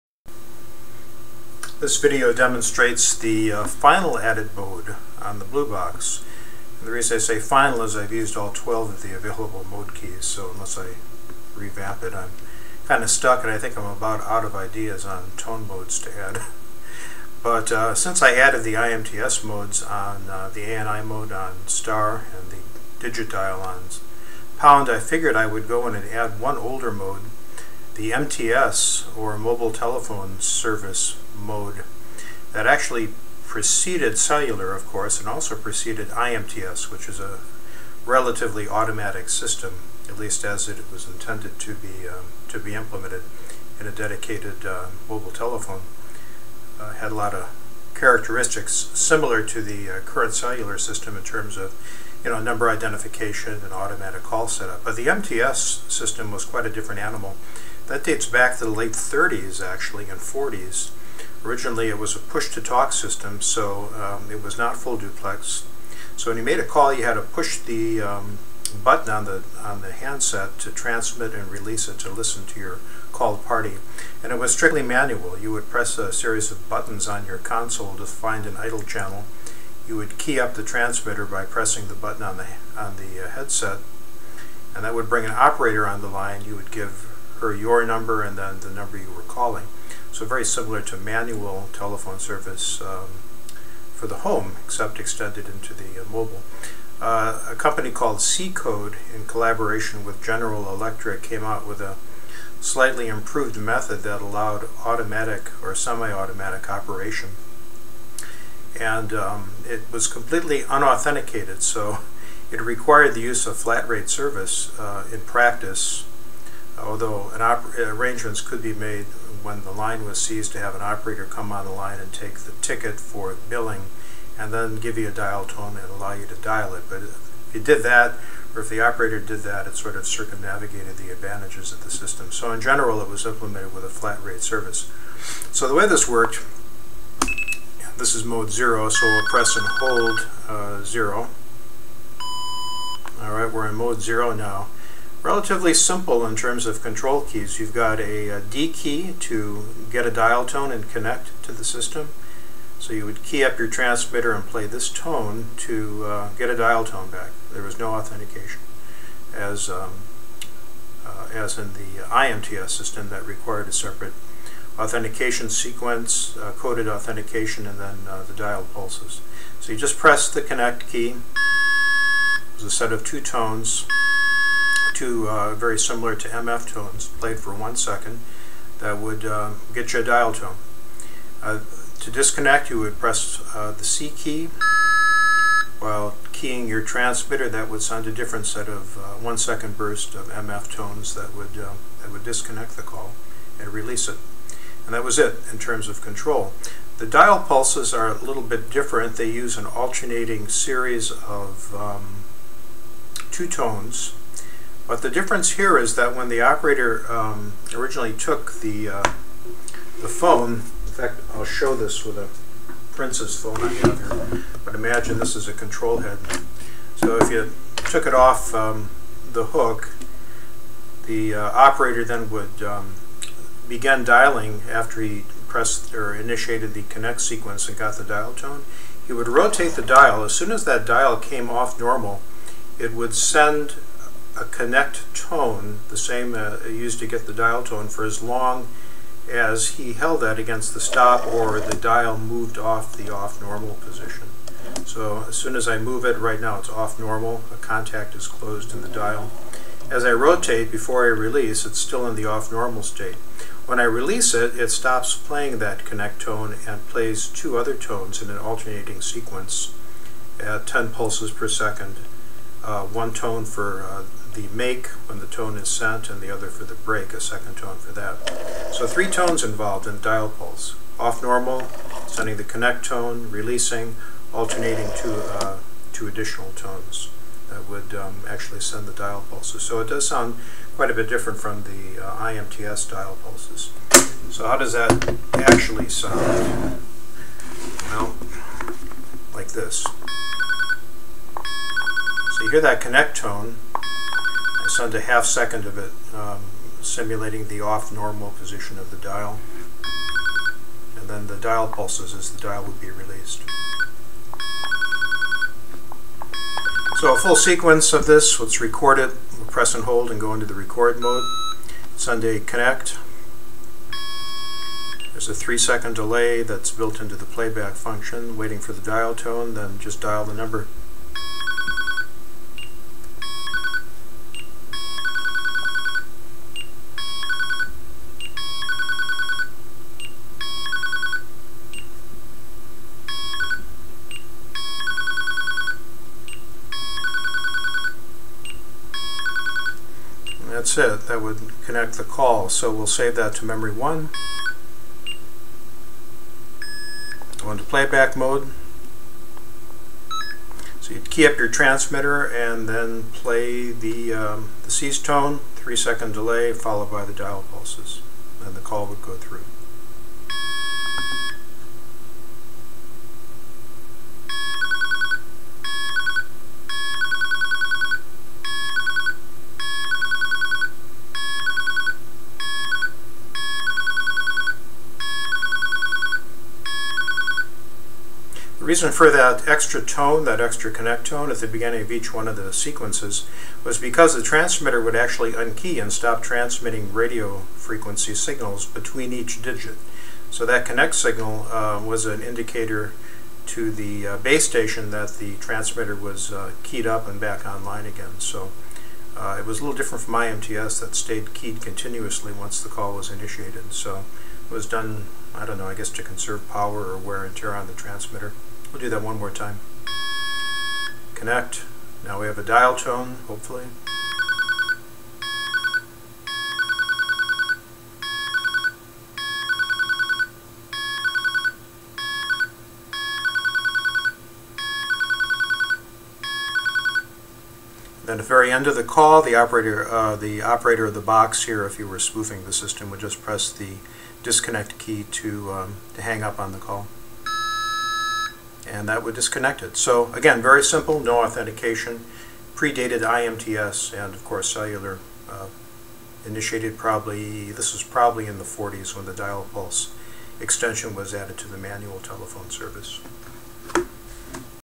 MTS (Mobile Telephone Service) "Dial" Mode (Secode/GE dialing system)
As long as I added the IMTS modes, I also added the earlier MTS (Mobile Telephone Service) improvement that added automated dialing to the manual system. This system did not support any authentication (as did the IMTS system, with ANI), so flat-rate service was required.

The use would send one tone to connect and get a dial tone, another to disconnect the call, and the unique dial pulses to send the dialed digits.

The transmitter unkeyed between dialed digits, so a third tone was sent when the user began to dial, moving the dial off its normal position.